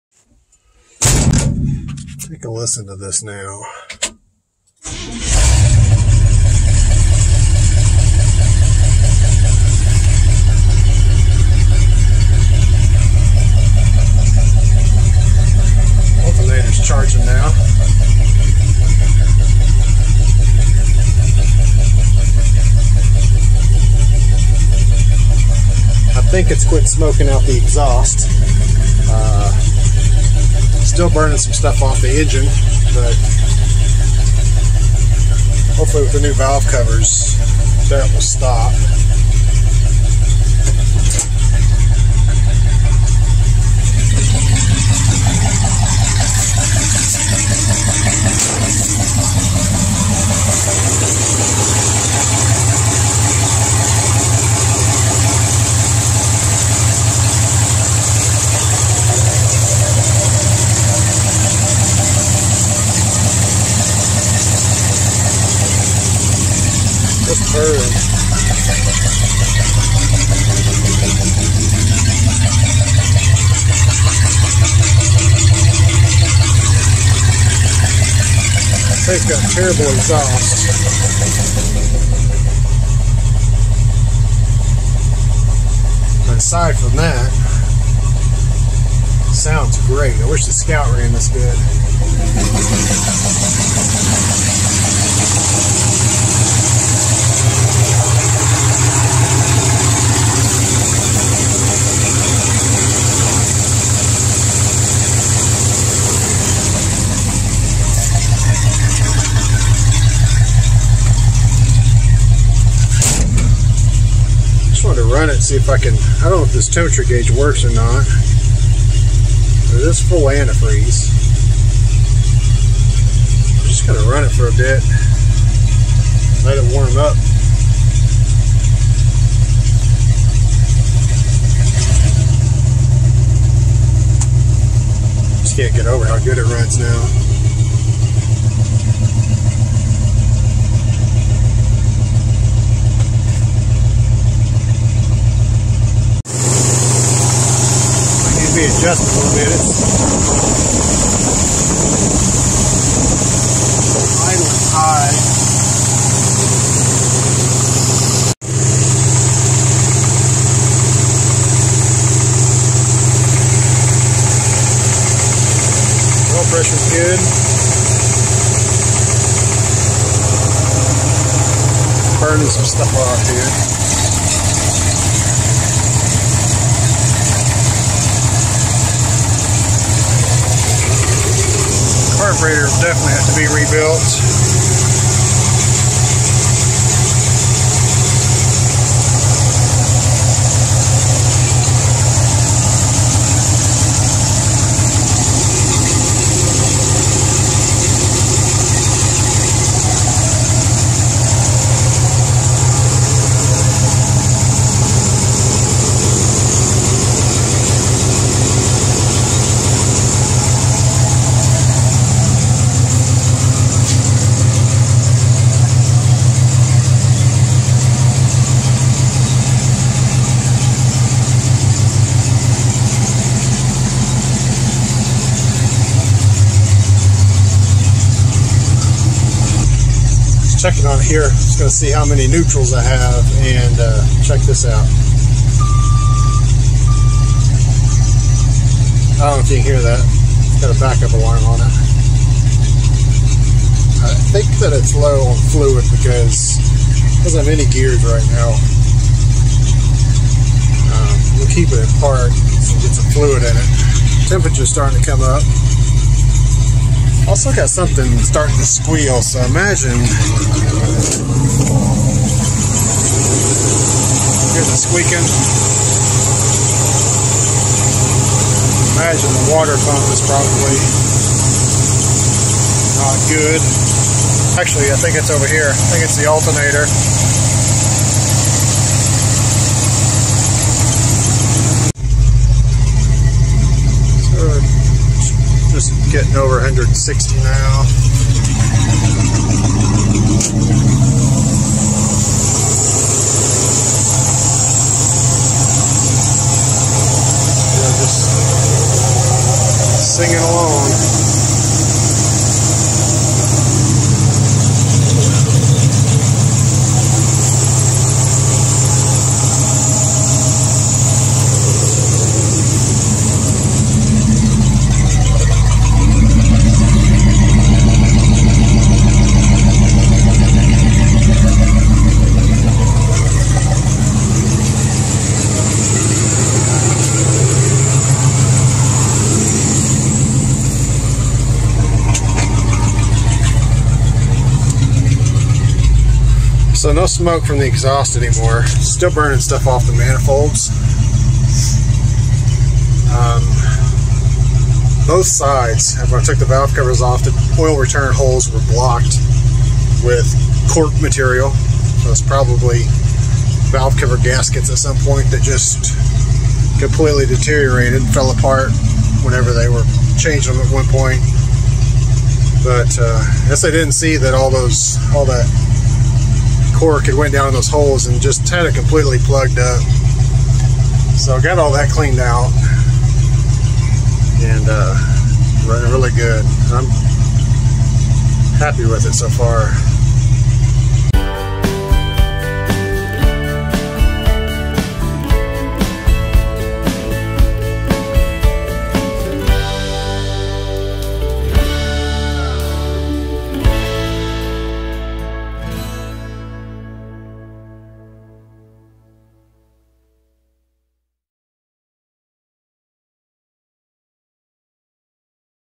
Full Temperature Run Up on the IH 345 V8
First run up to full operating temperature of the 345 V8 in the International Harvester 1210 pickup truck since sitting for a decade in a field and replacing a broken push rod.